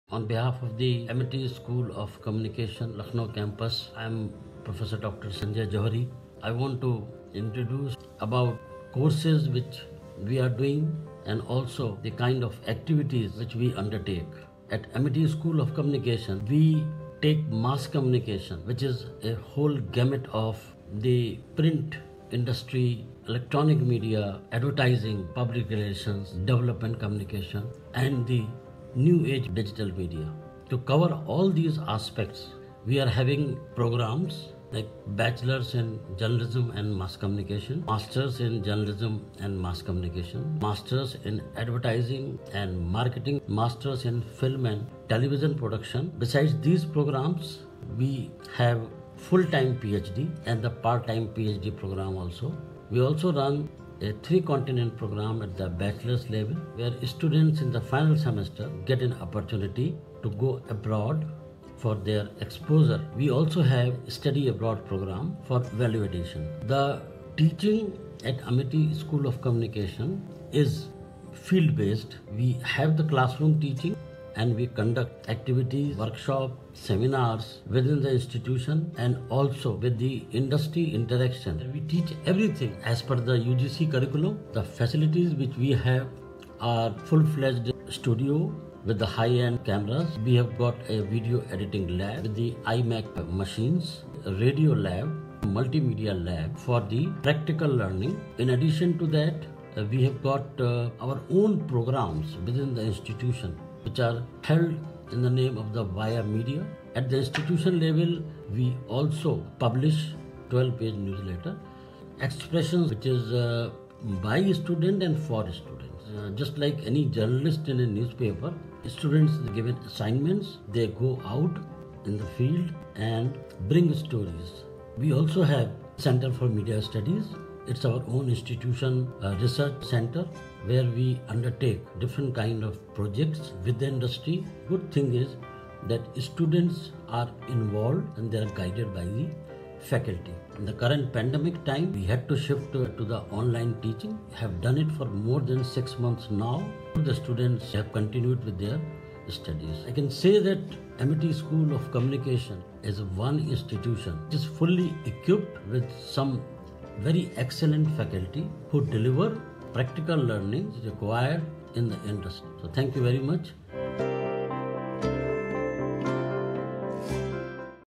Amity School of Communication at Amity University Lucknow
Prof. (Dr.) Sanjay M. Johri (Director, Amity School of Communication) speaks about the courses offered by AmityUniversityLucknow in the field of MassCommunication and the plethora of activities incorporated in the field-based teaching.